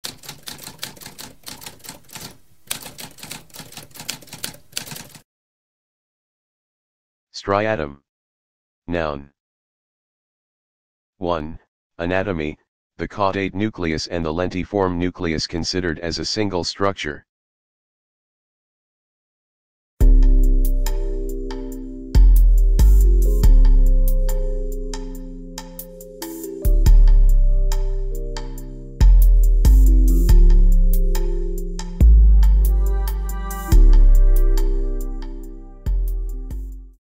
What does striatum mean?
A spoken definition of striatum.

Intro Sound:
Typewriter - Tamskp